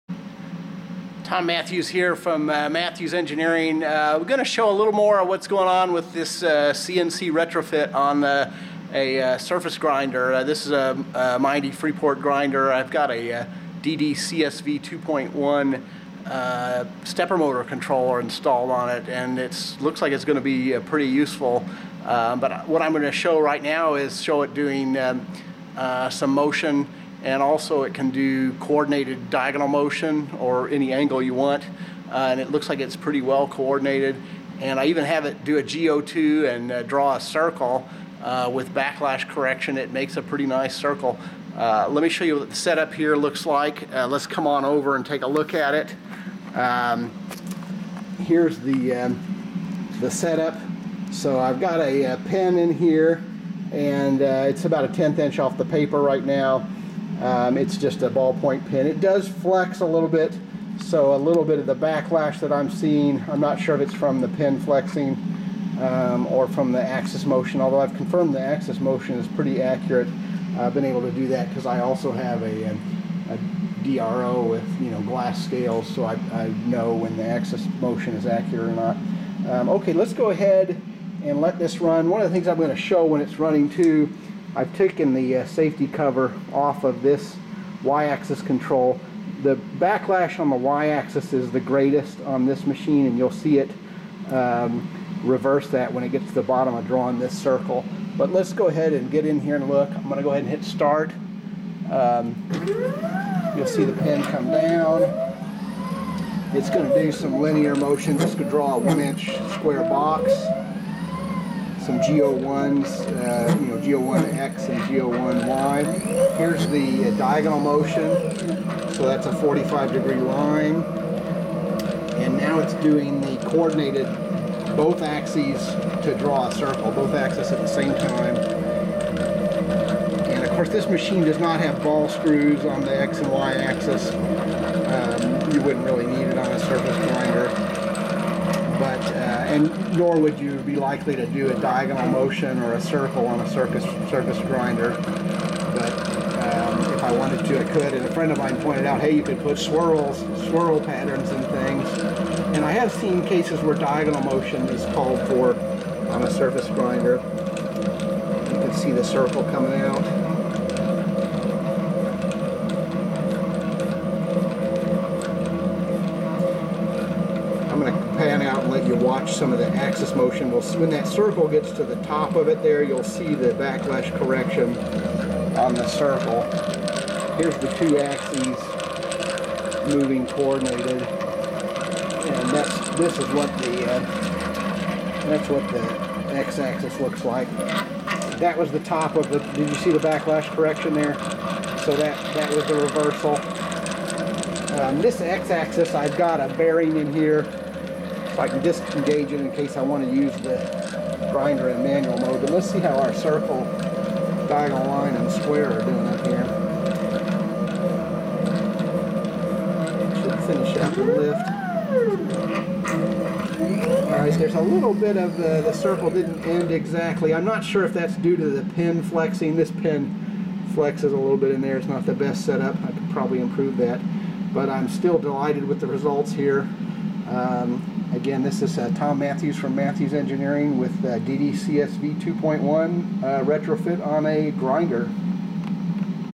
DDCSV2.1 G02 Circular Motion and G01 Coordinated Linear Motion
Coordinated linear motion and G02 circular motion on a DDCSV2.1 CNC retrofitted surface grinder.